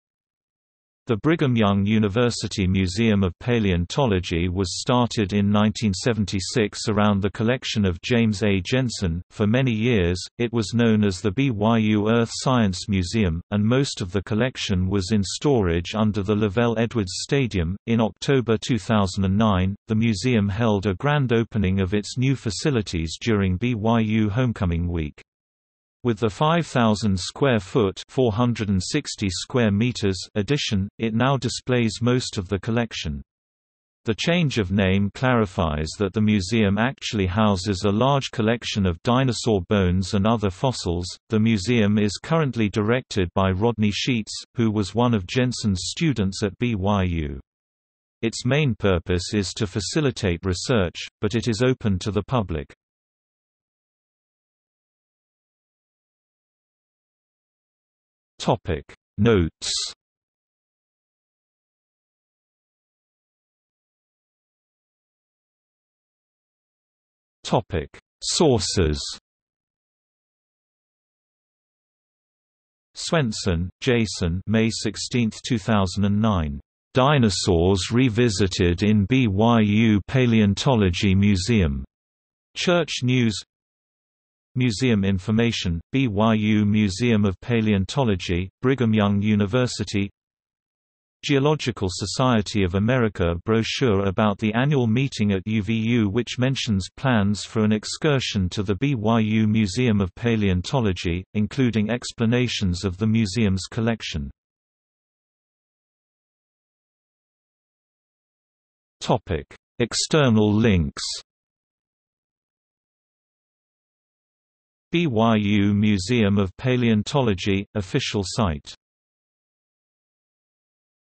BYU Earth Science Museum | Wikipedia audio article
00:00:54 Notes
00:01:03 Sources
00:01:45 External links

- Socrates

SUMMARY
The Brigham Young University Museum of Paleontology was started in 1976 around the collection of James A. Jensen.  For many years, it was known as the BYU Earth Science Museum, and most of the collection was in storage under the LaVell Edwards Stadium.In October 2009, the museum held a grand opening of its new facilities during BYU homecoming week. With the 5,000-square-foot (460 m2) addition, it now displays most of the collection. The change of name clarifies that the museum actually houses a large collection of dinosaur bones and other fossils.The museum is currently directed by Rodney Scheetz, who was one of Jensen's students at BYU.  Its main purpose is to facilitate research, but it is open to the public.